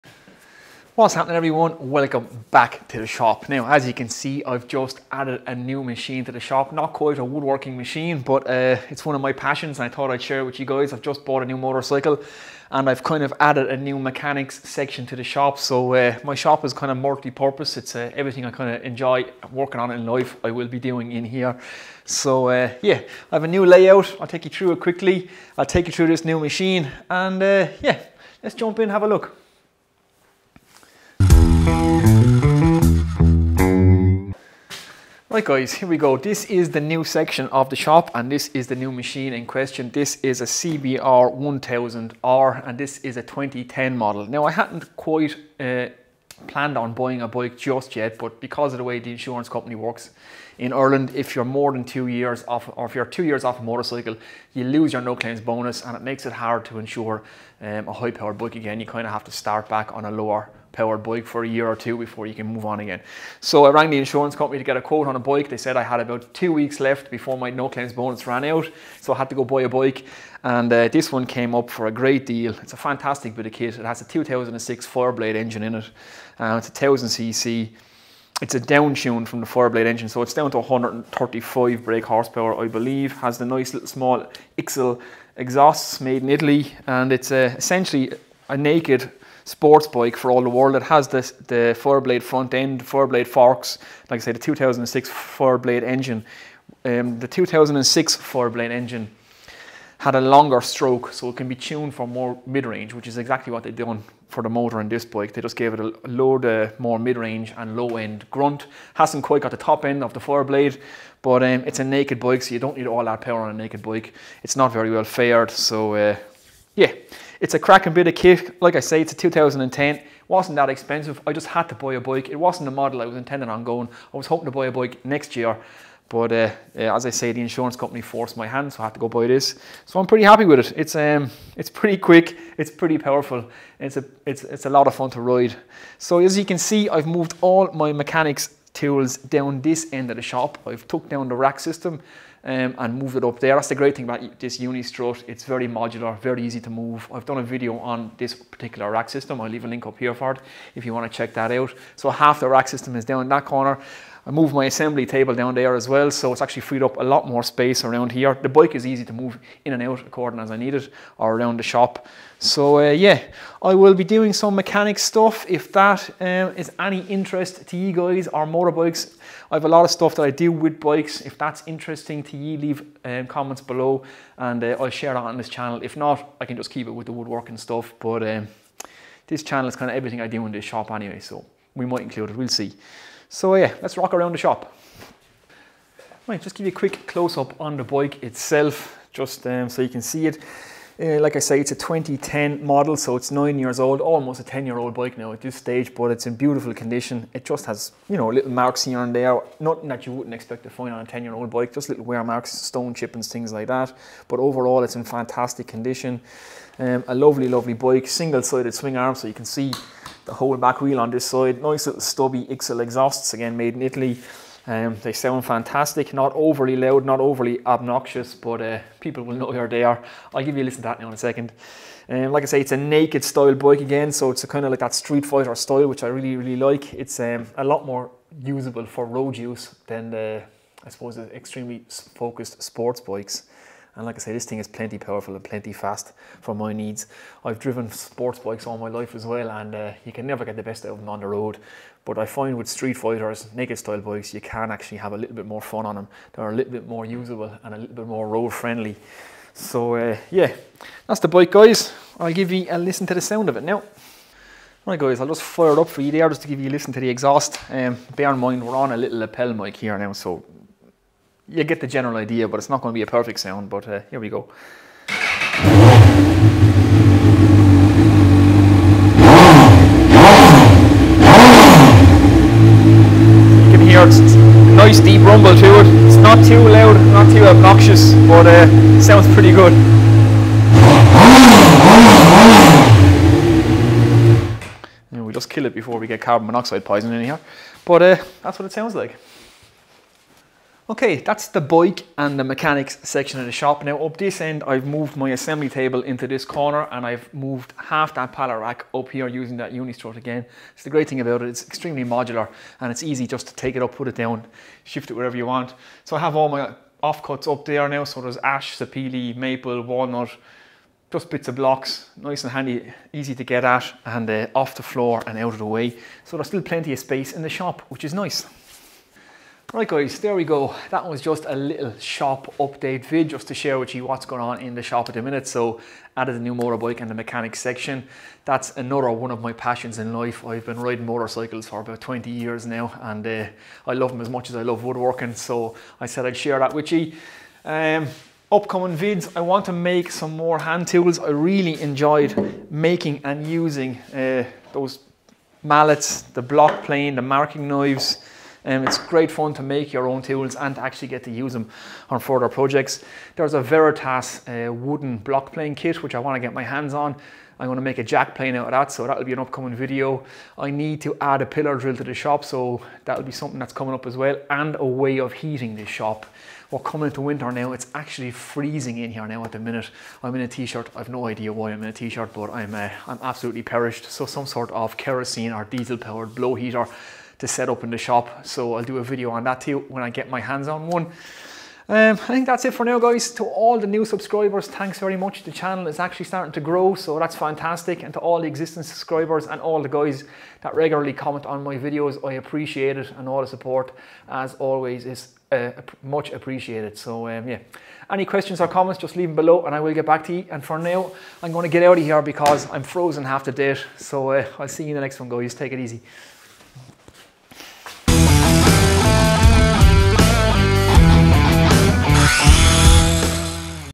New Machine Honda CB 1000R and Shop Update
Whats happening everyone, just added a new motorbike to the shop so thought I would do a quick vid just to share one of my other passions in life.